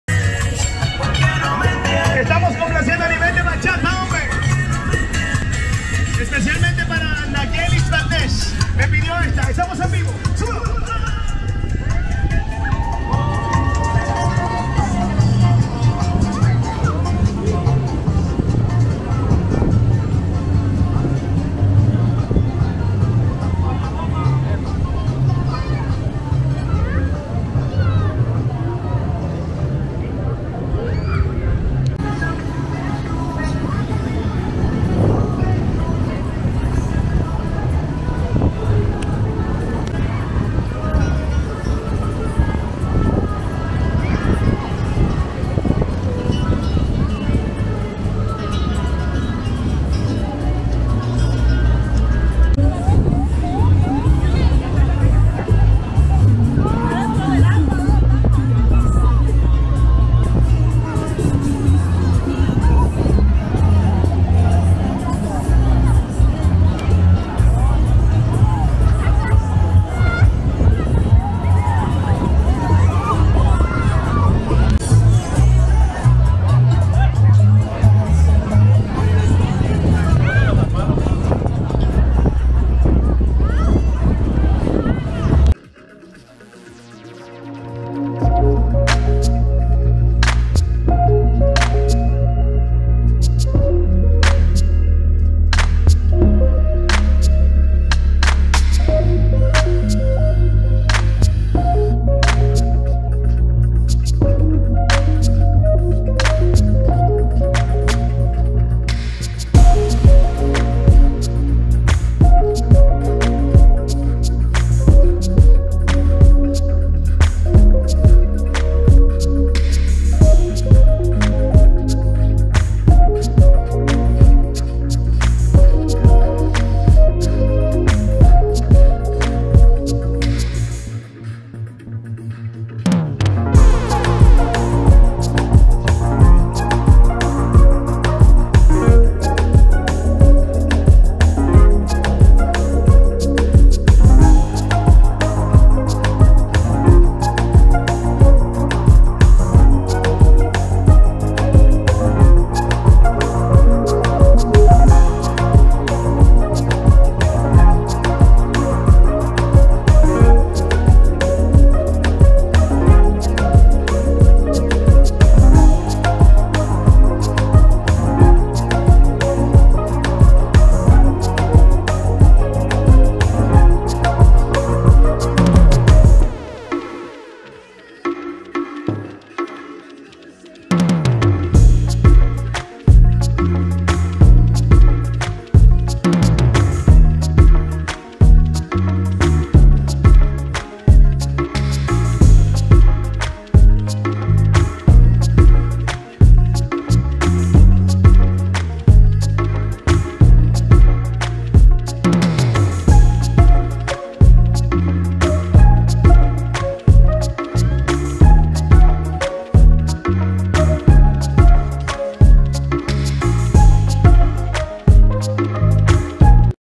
Michael Jackson impersonator Performs at Boca Chica Beach in the Dominican Republic.
One of the most popular and iconic pop stars of all time,  a Michael Jackson impersonator entertained thousands of people with his amazing dance moves and amazing voice. His music will forever be remembered and loved. Jackson was known for his incredible dancing skills, which his impersonator showed off at the Boca Chica Beach Dominican Republic . His performance was so amazing.